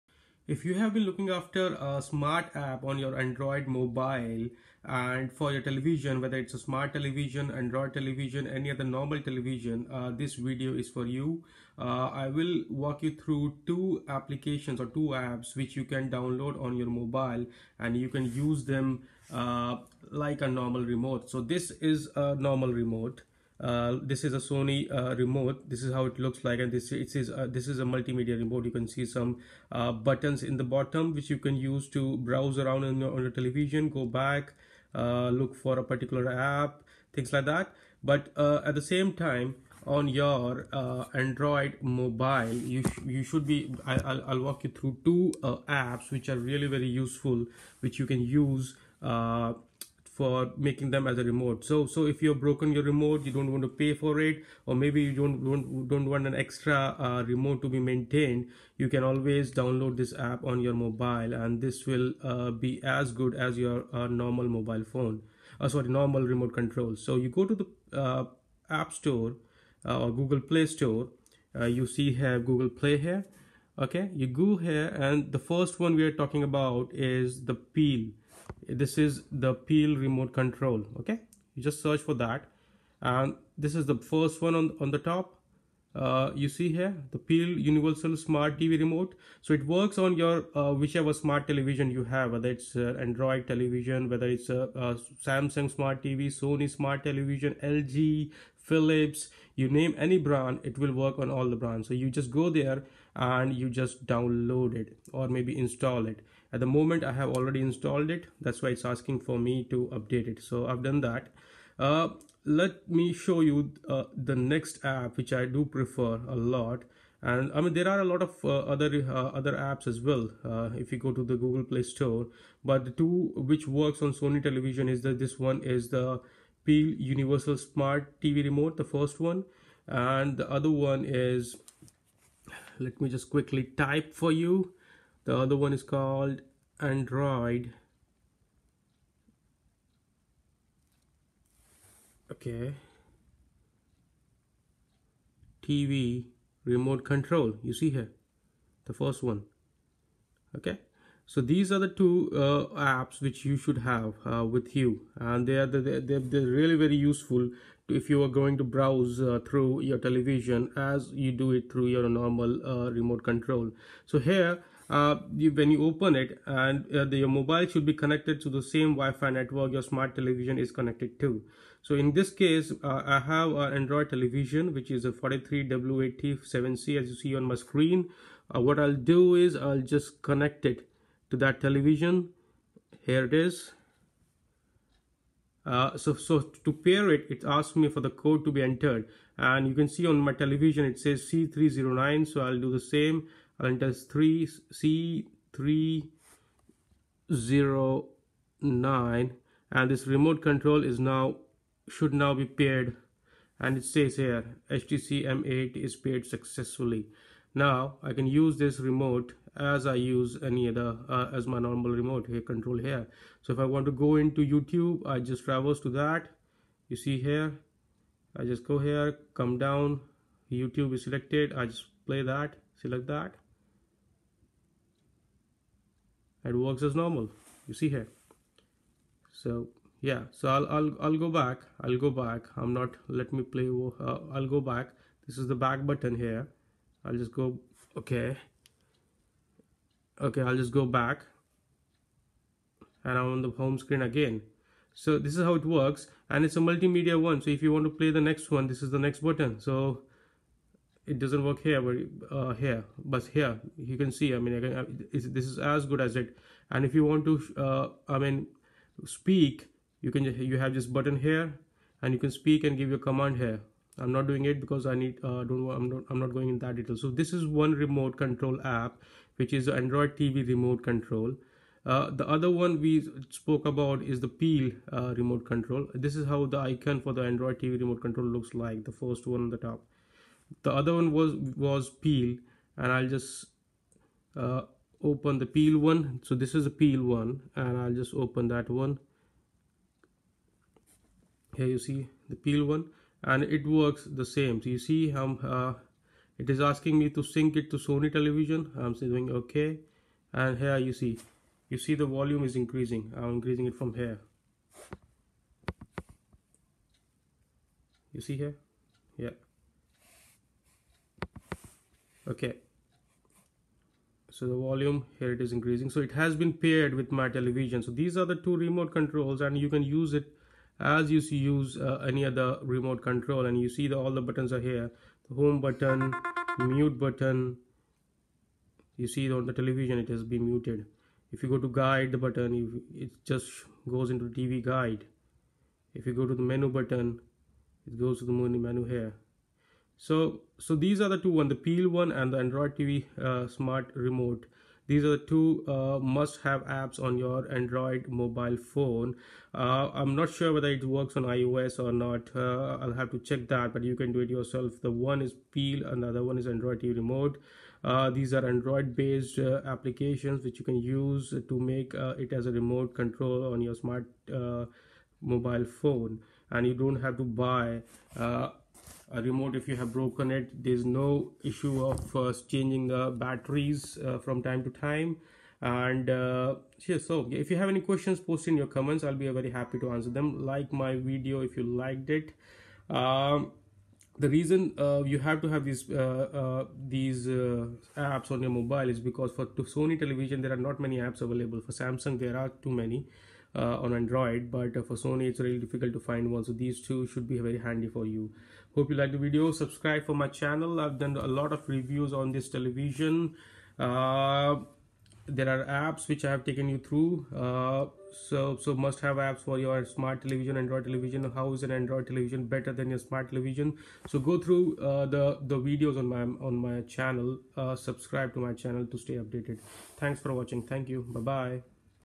Sony Bravia Android Smart TV Remote Control Android App | Peel TV Remote App | Smart TV Apps 2020
Shop Smart TVs and TV accessories from our Global stores:
United Kingdom
United States

Lets make full use of both these platforms.. We are growing !!

This is a MUST watch if you are after a good remote control app for your TV. Works  on on any smart tv, android tv or led tv. Sony TV , Samsung, LG Philipps or any other brand of smart television. This app is absolutely free and easy to install download and use.

Peel Smart Remote app allows you to use your phone or tablet as an advanced TV remote control. In addition to flicking through channels, you gain access to cable & local television programming with cover art thumbnails, look ahead to upcoming programming, set reminders to watch and save favorites, plus extend the conversation on social media. Watch this video review to see why you'll enjoy using this app!